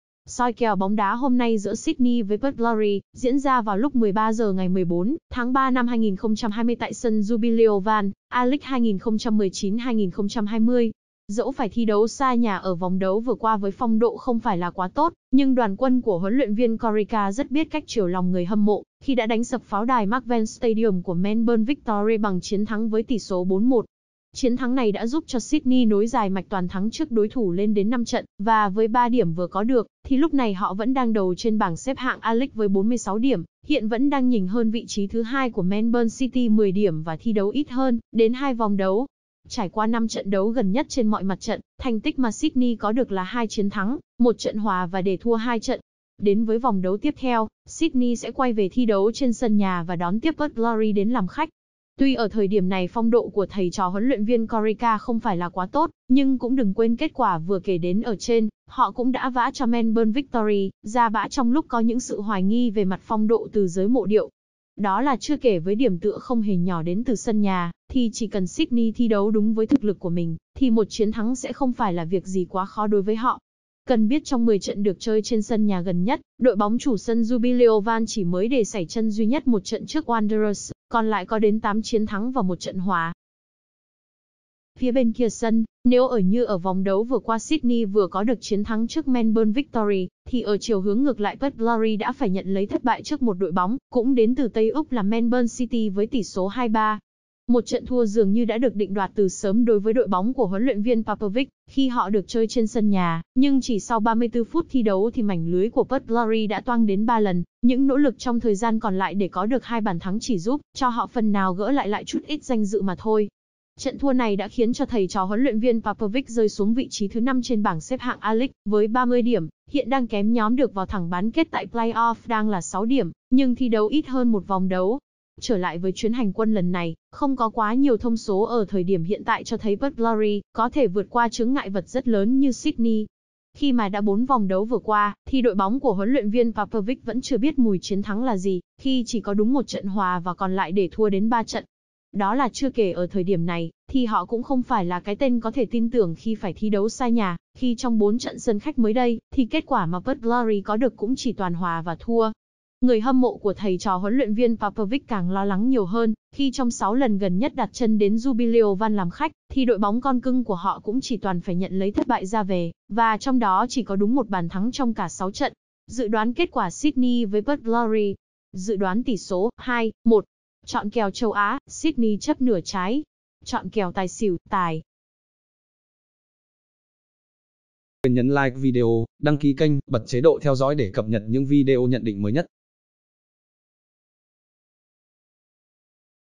Sydney vs Perth Glory 13h00 ngày 14/03/2020 - vòng 23 A League 2019/2020
Soi kèo bóng đá hôm nay Sydney vs Perth Glory diễn ra vào lúc 13h00 ngày 14/03/2020 tại sân Jubilee Oval, A League 2019/2020.
Thưởng đối đa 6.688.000 khi đăng ký chơi thể thao tại